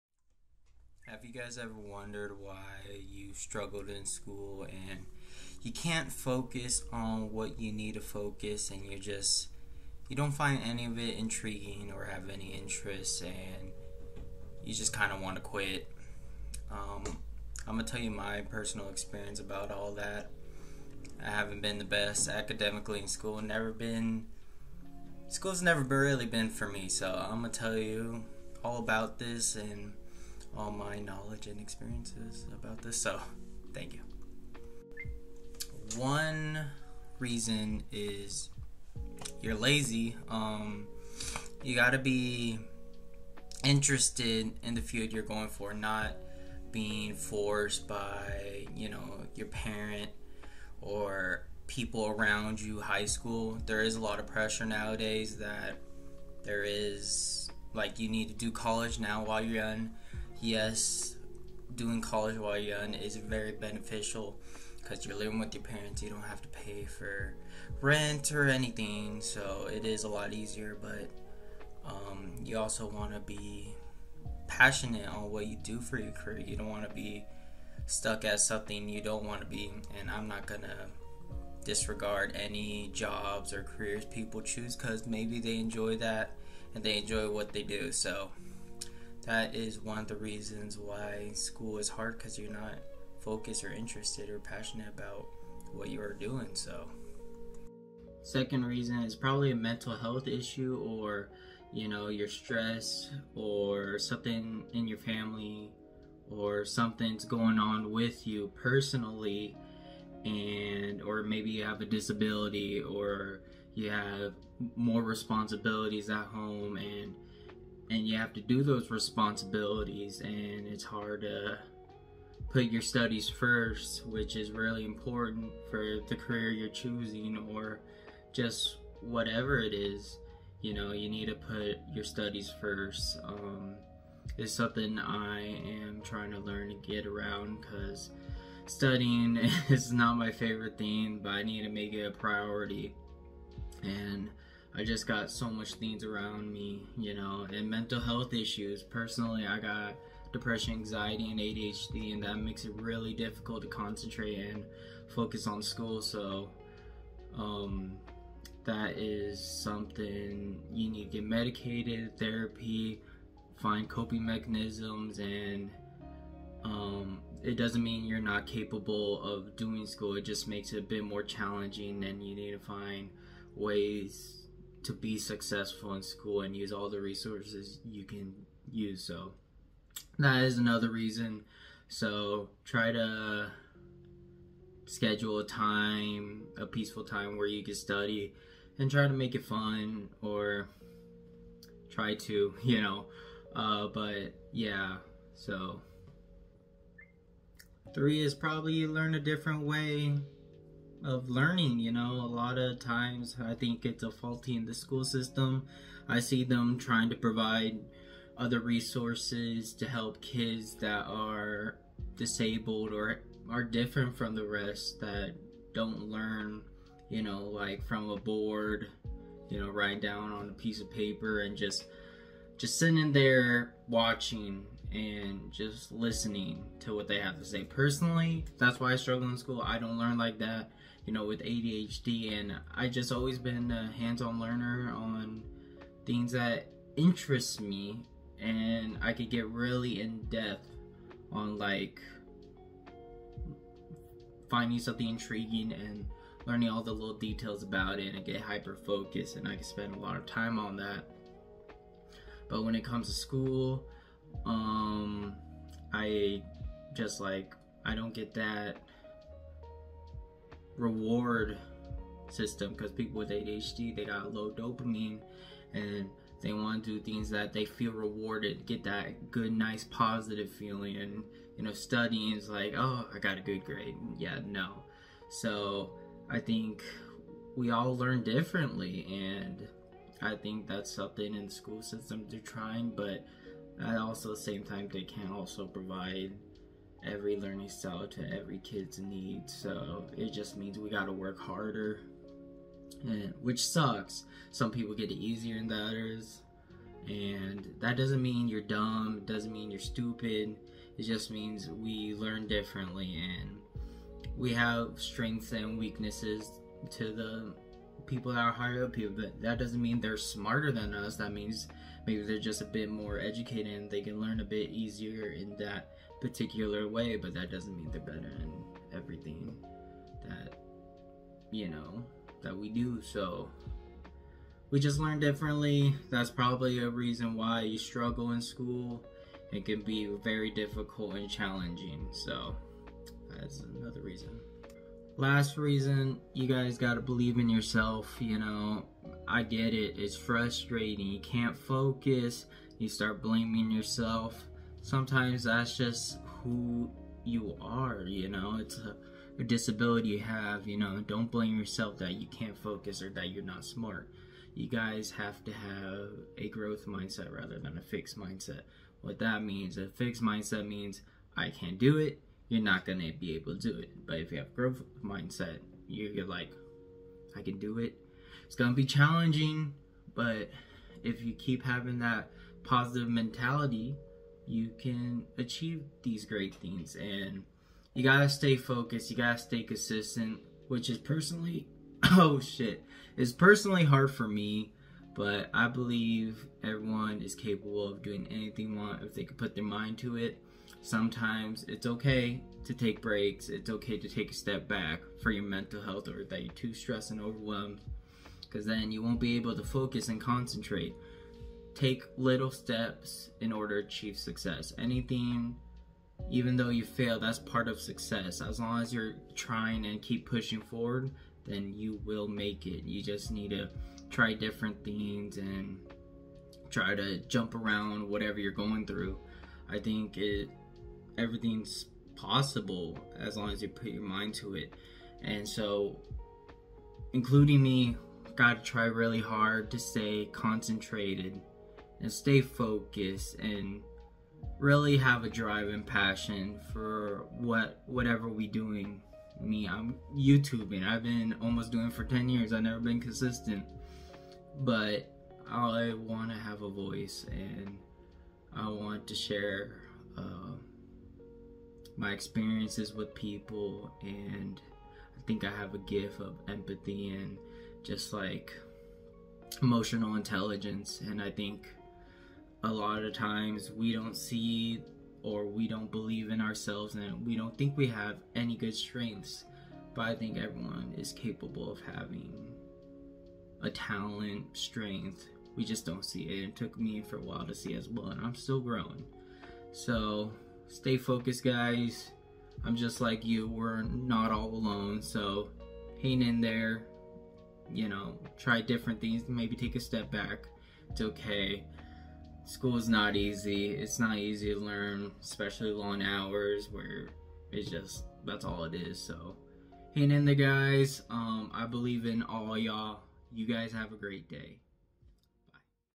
Why is school so difficult?
Hope you guys enjoyed going to be making video like this with advice to help you through life while sharing my experiences, knowledge, and opinions about many things.

0:00 Introduction
0:35  You are lazy
1:38  Mental health
3:17 Different learning style
5:35 Believe in yourself